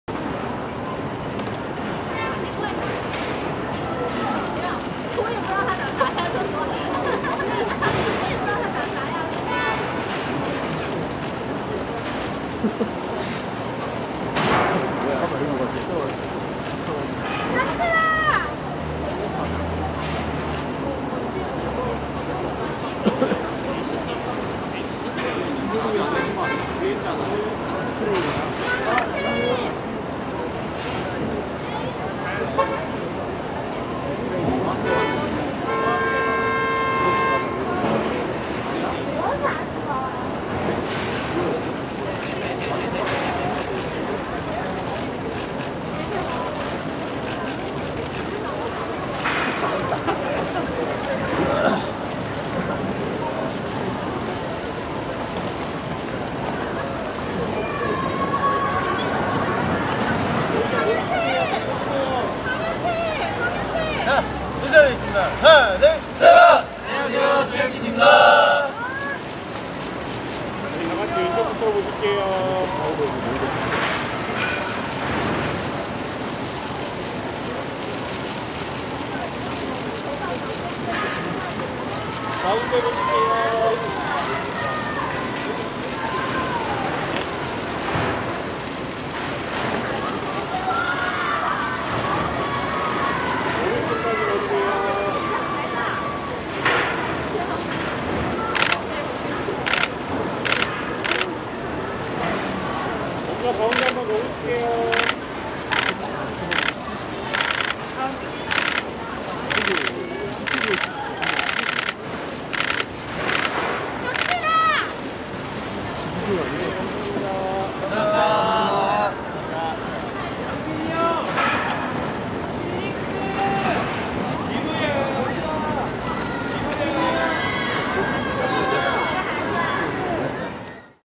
Stray Kids (스트레이 키즈) @ 190329 뮤직뱅크 출근길
29일 KBS 뮤직뱅크에 스트레이키즈가 출근하고 있다.